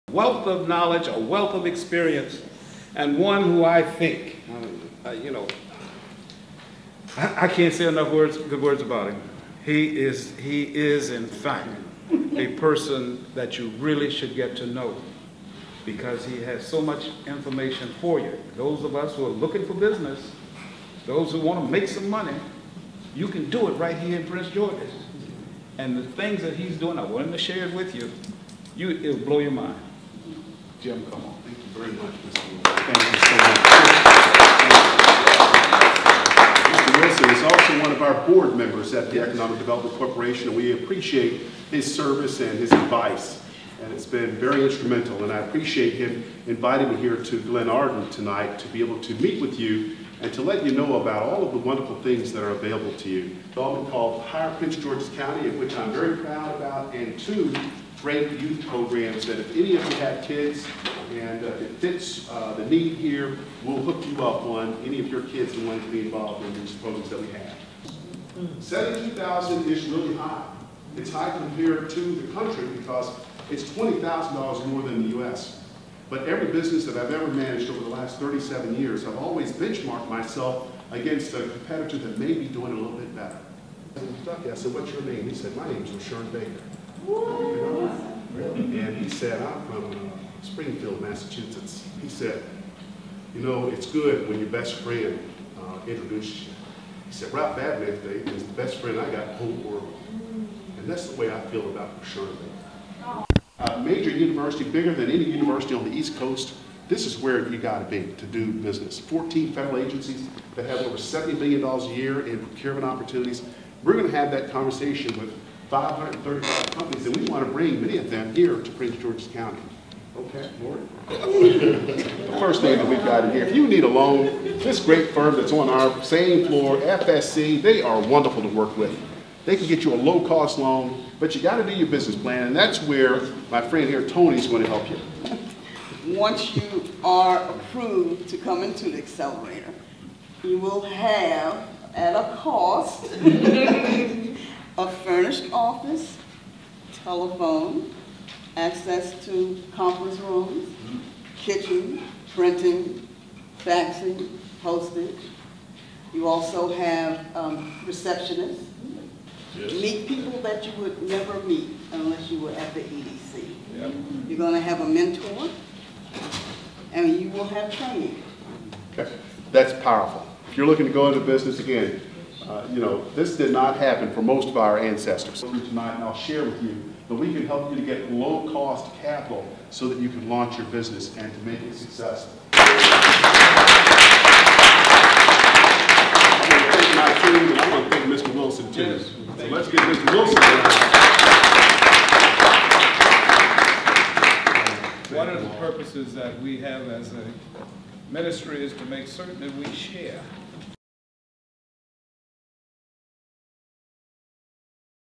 EDC President Jim Coleman Speaks at First Baptist of Glenarden, CIB Ministry
On Friday, October 9, 2015, Jim Coleman spoke fervently to local business owners at the FBCOG's Christian Business Institute’s monthly meeting. Coleman shared how the EDC is facilitating the attraction, retention and expansion of major employers using tools such as: the County’s $50 million Economic Development Incentive Fund; the “We Care” and “Operation 535” EDC initiatives; tax and financial incentives; and EDC’s workforce development and training programs.